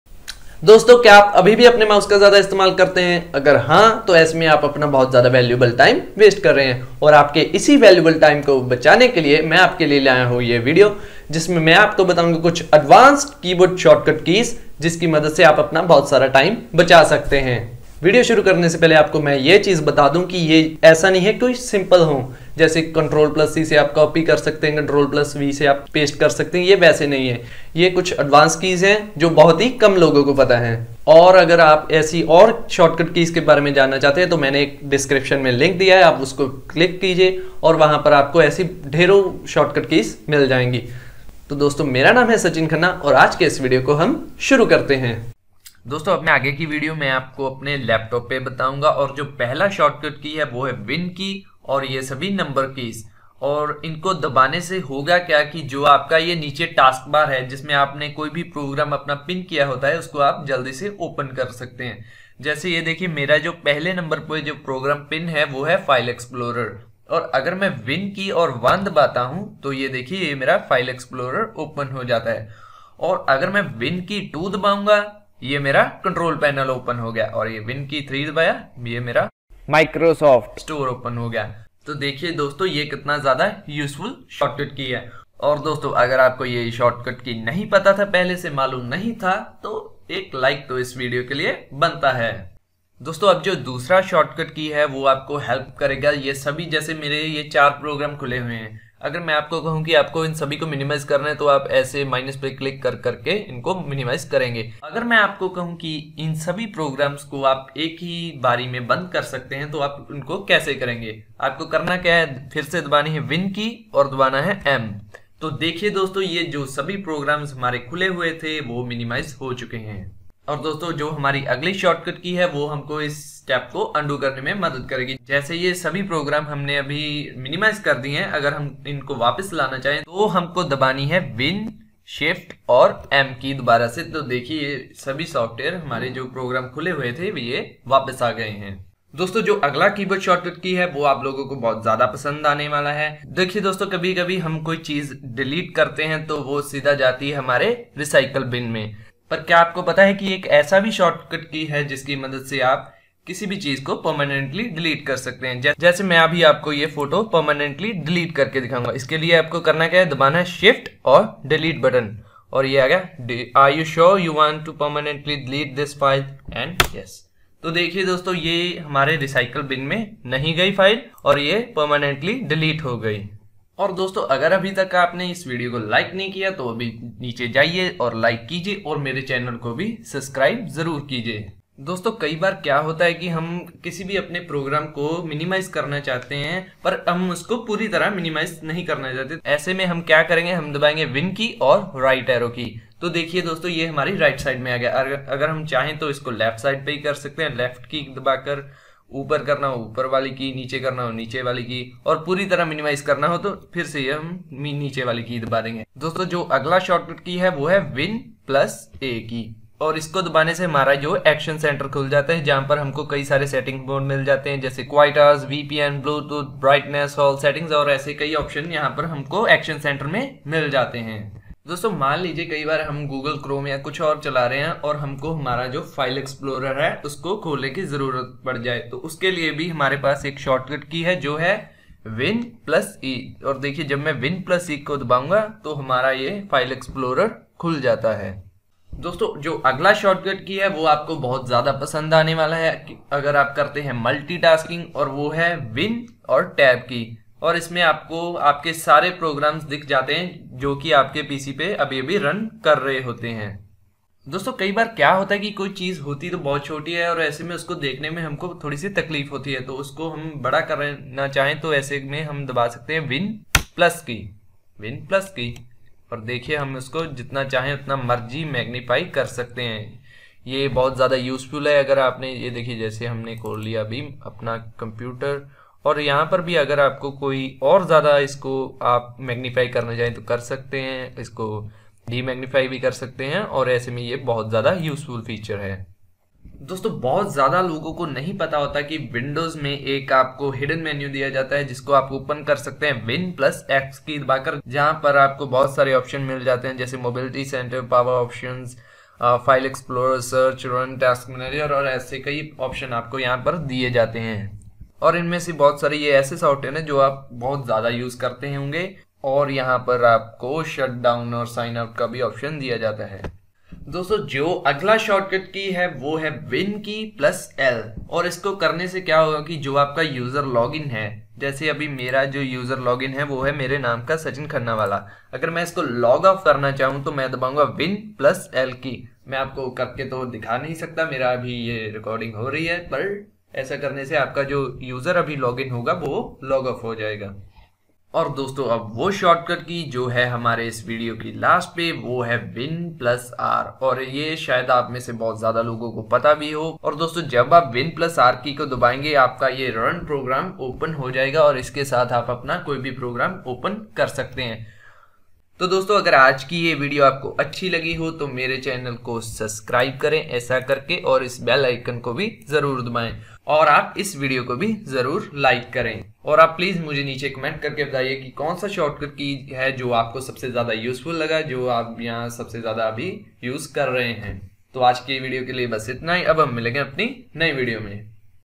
Advanced Keyboard Shortcut Keys: [Windows 10] - (May. 2020)
In today's video, I've shown you some advanced keyboard shortcut keys for Windows 10, 7, etc. Most of these keyboard shortcut keys are not known and very few people know them.

And after watching this video you'll be one of them. And these keyboard tricks will definitely help you to work faster.

Dosto aaj ke iss video main maine aapko kuch aadhunik keyboard shortcut keys ko bataya hain. Aur dosto iss video ko jarur like karen aur share bhi karen.